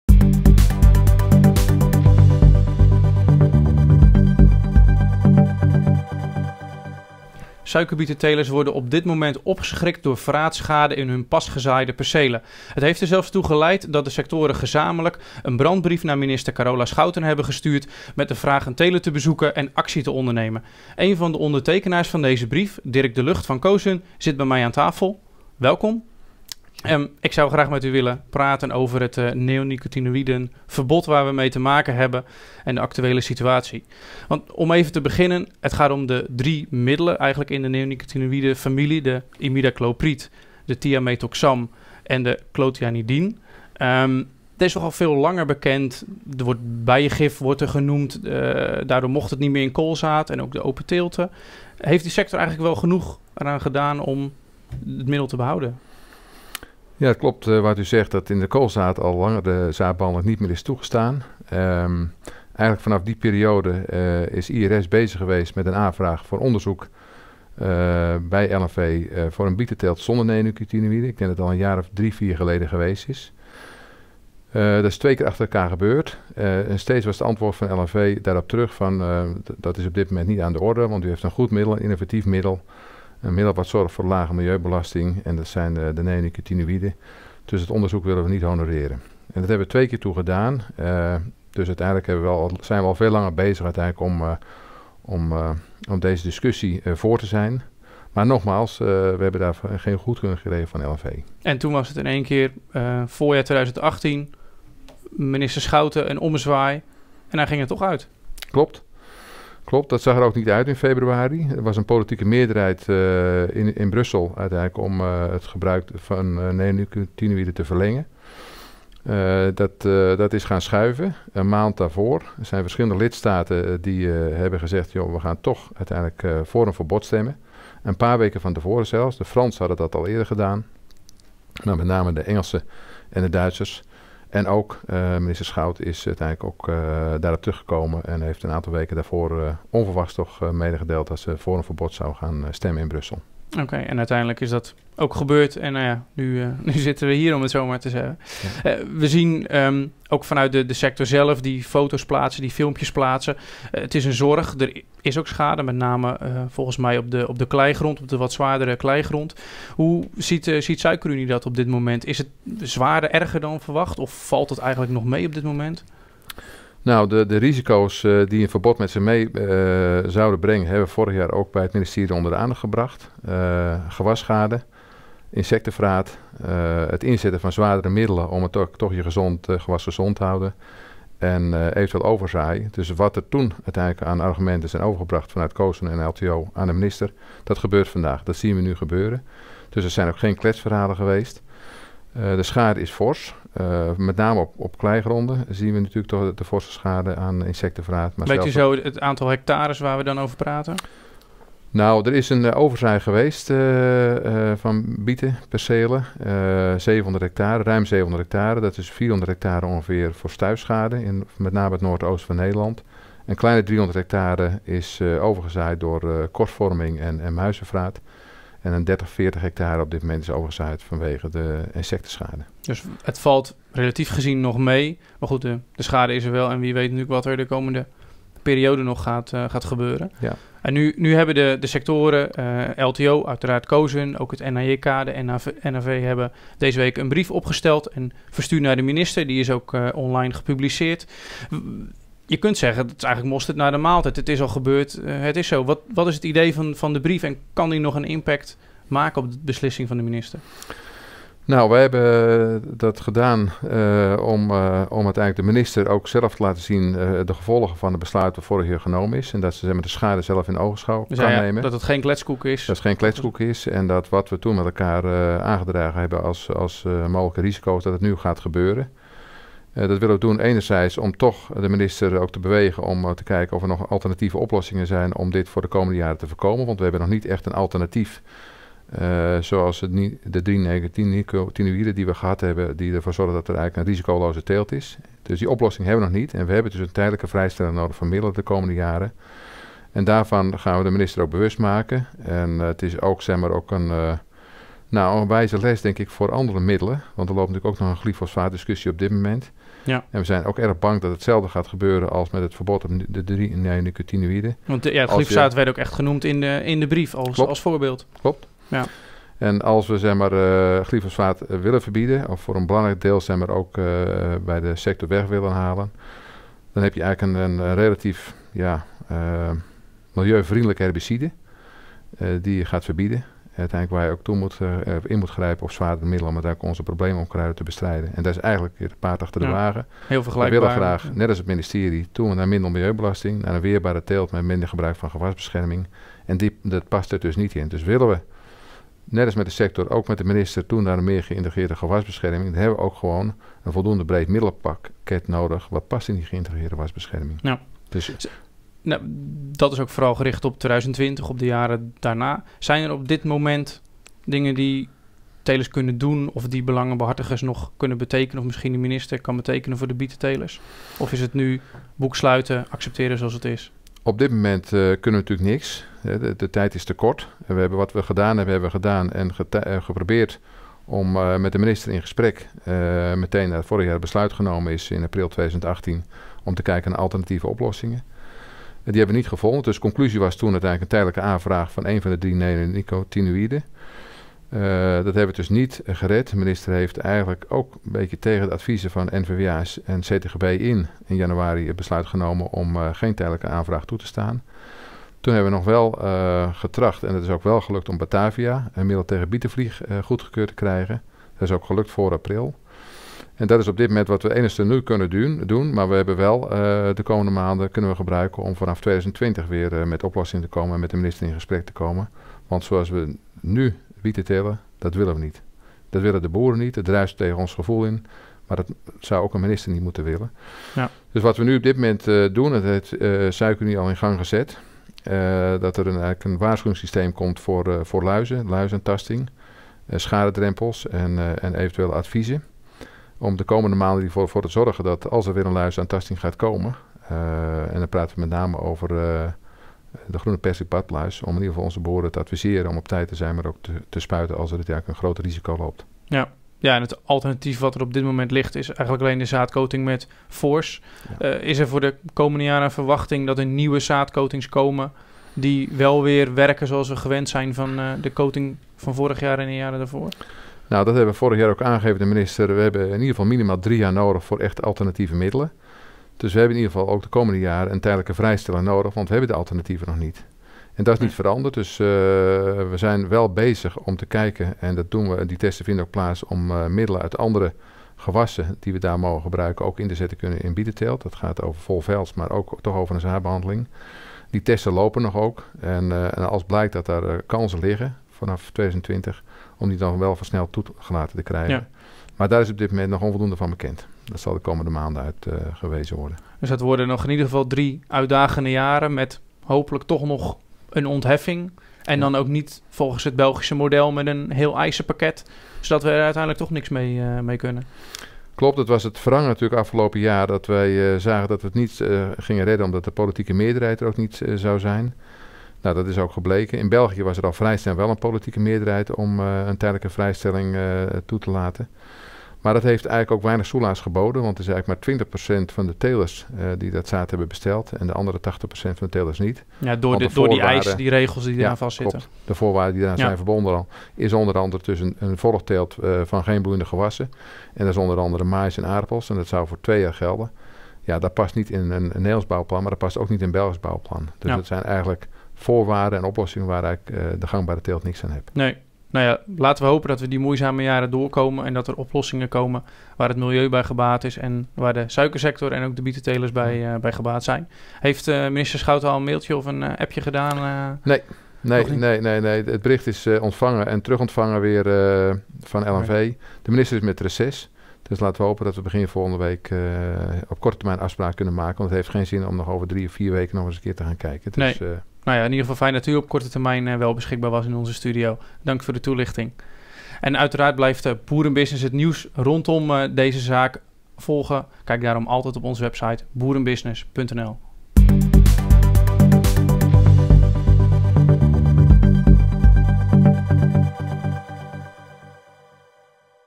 BB TV: Dirk de Lugt - 'Zoals we nu bieten telen, willen we niet'
De jonge suikerbietengewassen worden geteisterd door insectenvraat. Het is een direct gevolg van het verbod op de zaadcoating met neonicotinoïden. De sector klopt aan bij minister Carola Schouten (Landbouw, Natuur en Voedselkwaliteit) en vraagt haar de gevolgen met eigen ogen te zien. "Zoals we nu bieten telen, dat willen we niet", zegt Dirk de Lugt, voorzitter van Cosun.